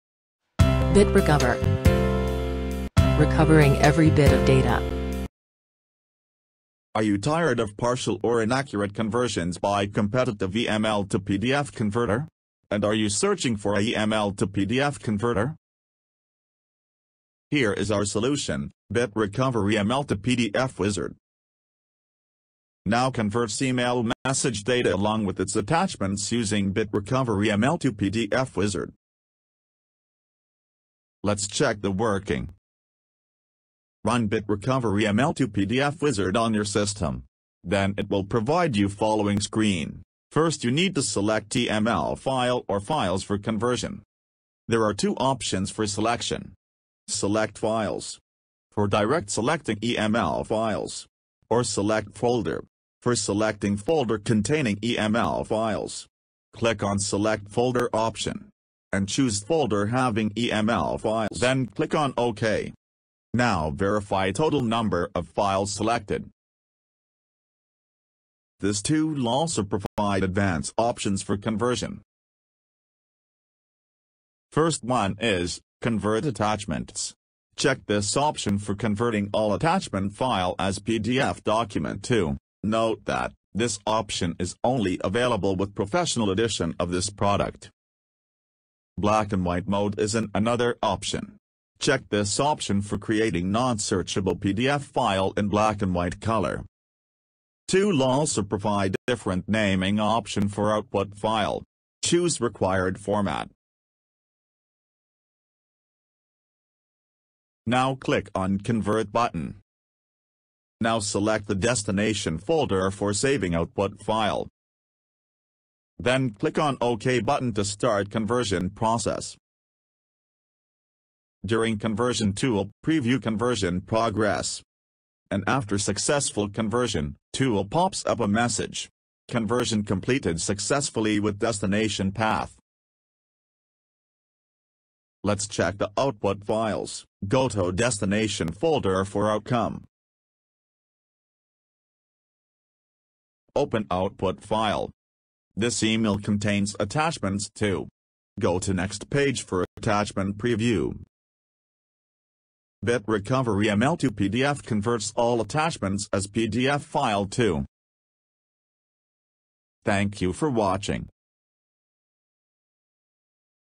Convert EML to PDF with Attachments as PDF too
Learn, How to Convert EML to PDF with Attachments as PDF too. Now convert Batch EML to PDF document with attachments as PDF, using BitRecover EML to PDF Utility. Tool also provide different naming options for output PDF files.